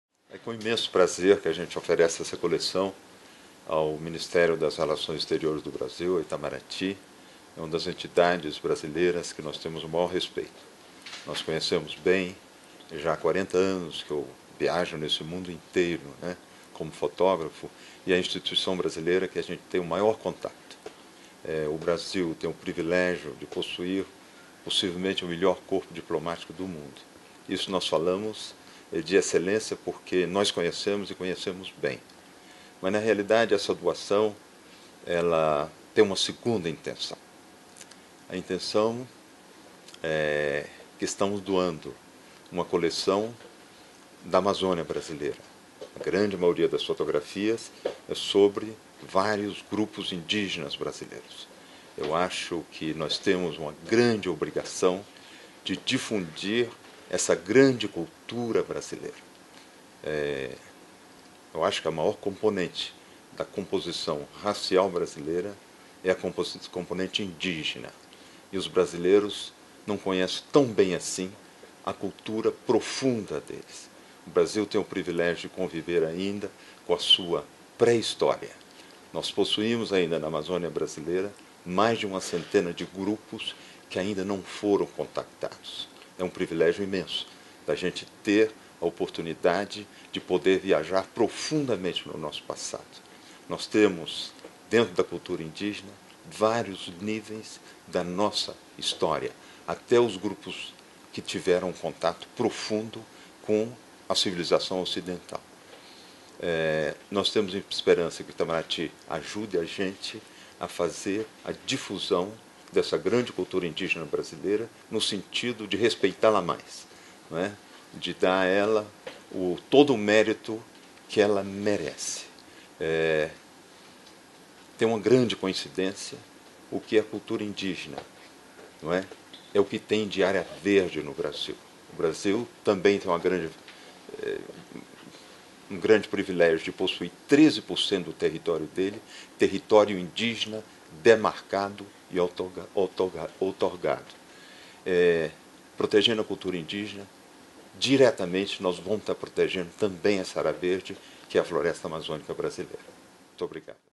Depoimento - Sebastião Salgado e Lélia Wanick Salgado
O fotógrafo Sebastião Salgado e a curadora Lélia Wanick Salgado compareceram hoje ao Itamaraty para efetivar doação de 24 obras ao acervo do Ministério. Em entrevista ao canal do MRE no YouTube, o fotógrafo falou sobre sua carreira, a colaboração prestada pelo Itamaraty a diversos de seus projetos e a importância da temática indígena em sua obra. Gravado em Brasília, em 2 de setembro de 2014.